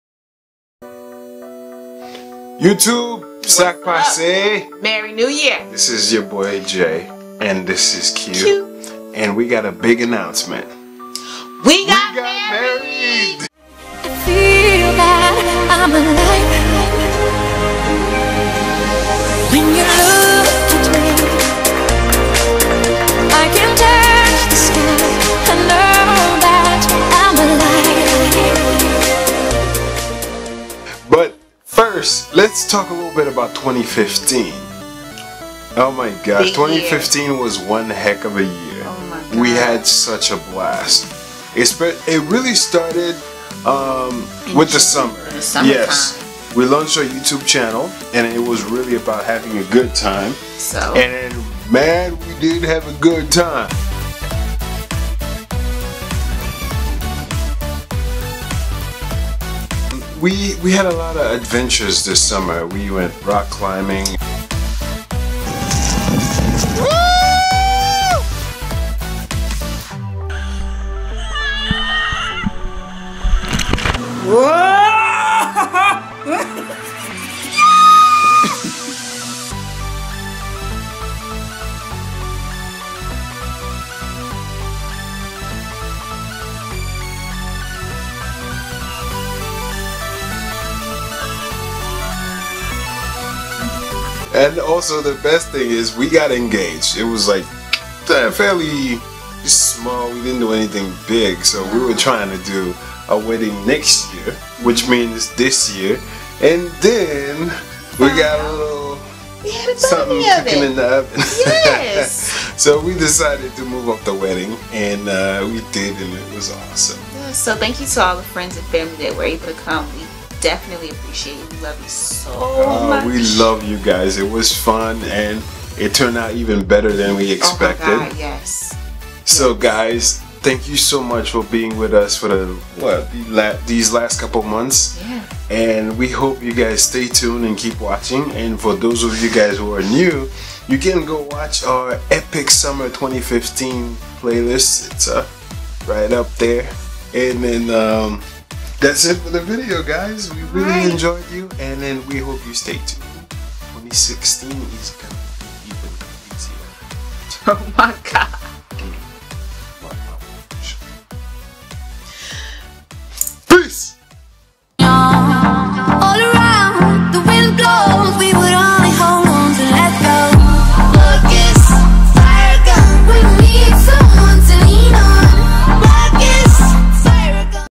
WE GOT MARRIED!! GOodbye 2015 it's been THE BEST!
OMG 2015 WAS THE BEST YEAR EVER.
It was fun doing a recap of all the fun things we did.
We had an epic summer. Got engage, married and now a baby.
Talking about one heck of yearrrrrrrrr

AND BECOME A "CHRONICLER"

JAwesOOme
Chronicles_Kew